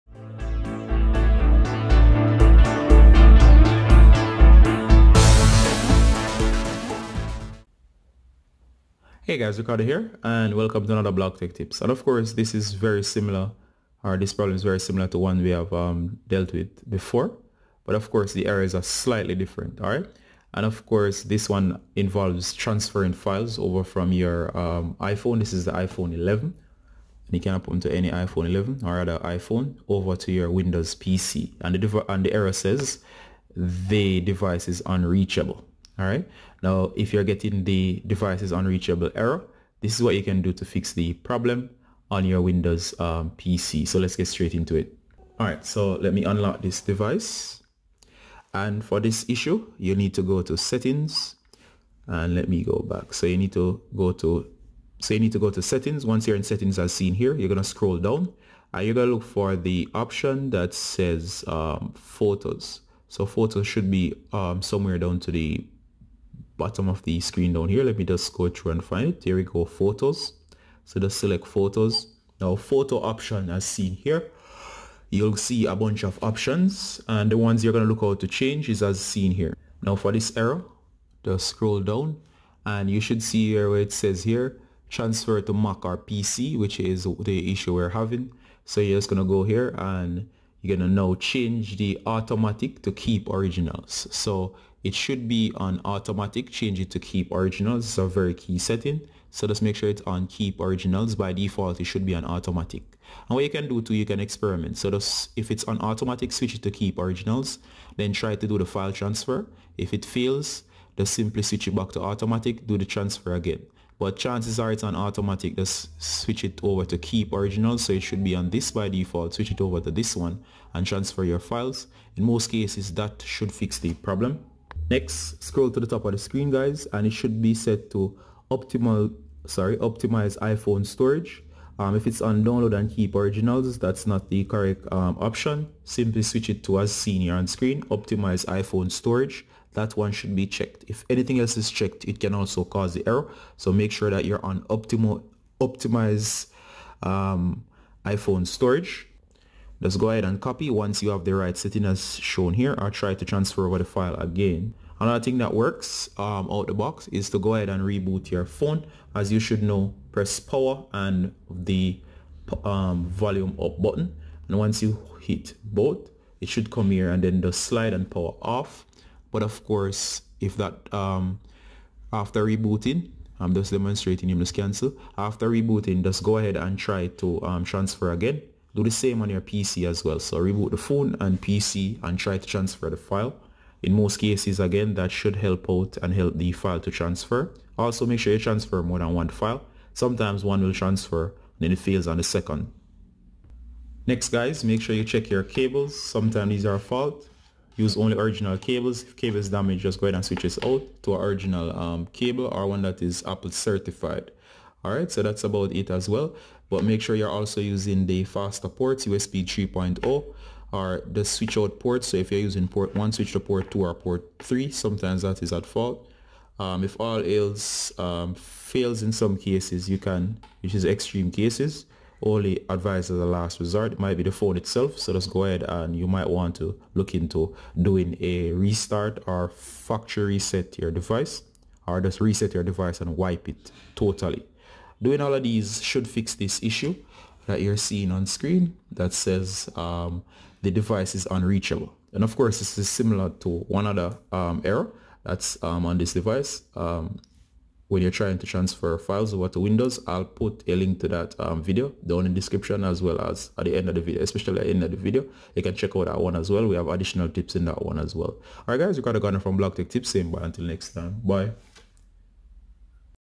Fix The Device is unreachable on iPhone when transferring files to Windows
If you are trying to transfer files from your Windows Computer to your iPhone and getting: The Device is unreachable then this will fix the problem 100%. Easily transfer your files as normal from your iPhone.